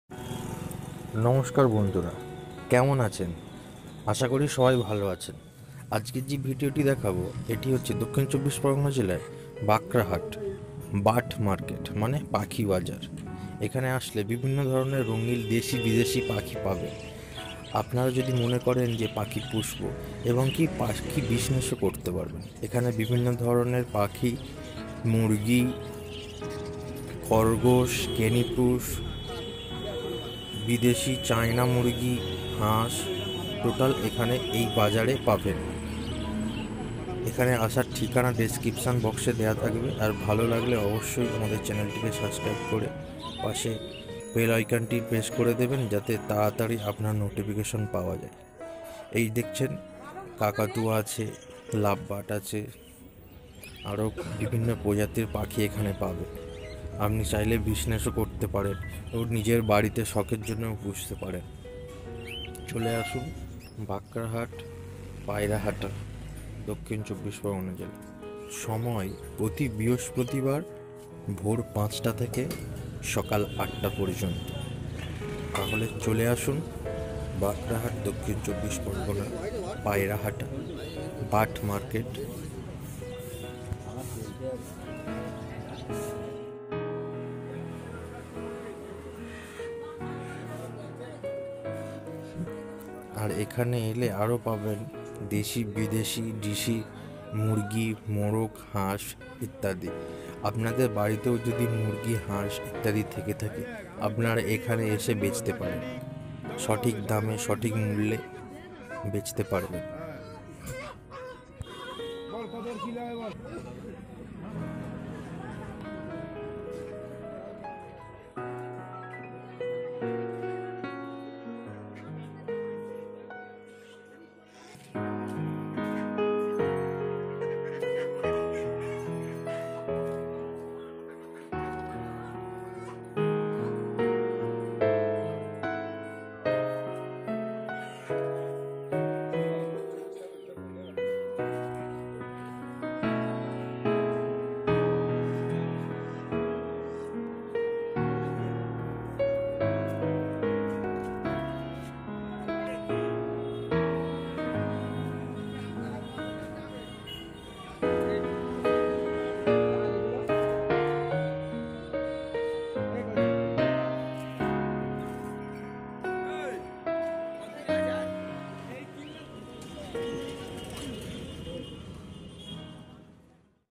love birds এবং দেশি মুরগির সেরা হাট Low price in Kolkata Bakhrahat
বাখরাহাট পায়রা হাটা নিবারণ দত্ত রোড দক্ষিন ২৪ পরগনা
Adventure with Bente
আরো ওনেক মজার মজার ভিডিও পেতে আমাদের সঙ্গে থাকুন